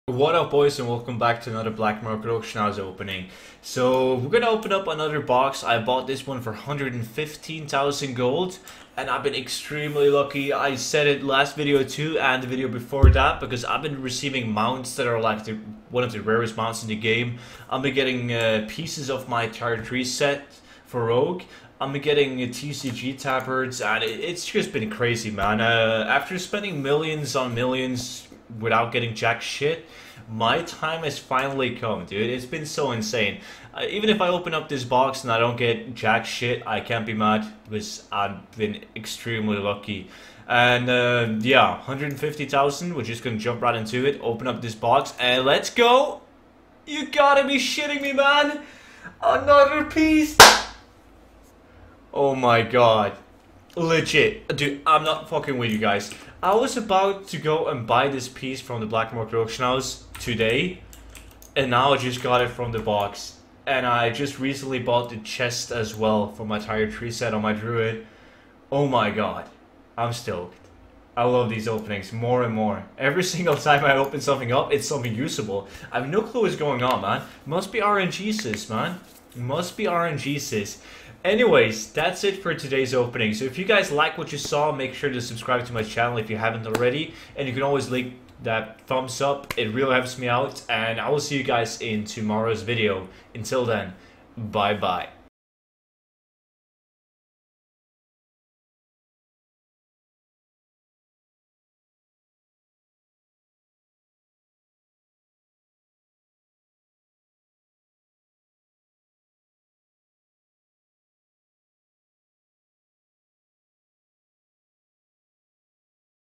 Opening Unclaimed Black Market Container #20
I Made an E-Book on How to achieve GOLDCAP! Opening an Unclaimed black market container bought on the black market auction house, let's get some mounts!